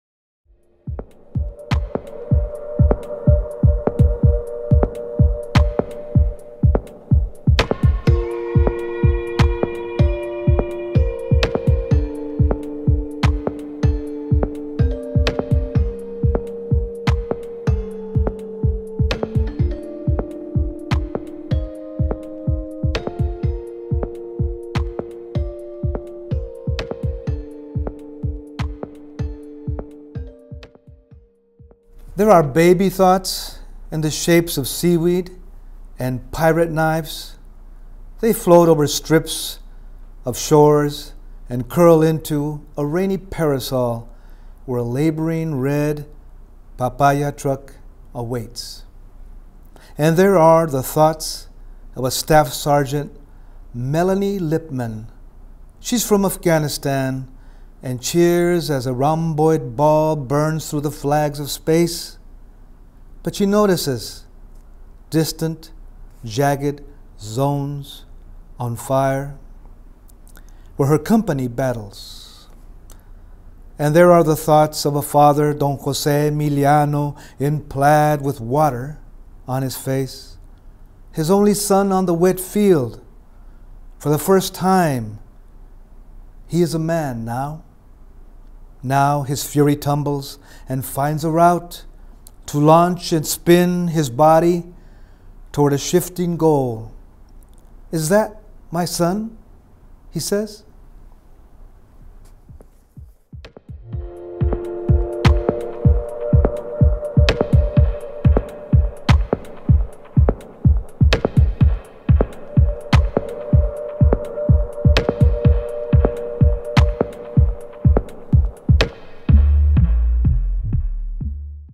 Juan Felipe Herrera: Dear Poet 2016
Juan Felipe Herrera reads his poem “All the Thoughts at a Football Game” as part of Dear Poet, the Academy of American Poets' educational project for National Poetry Month 2016.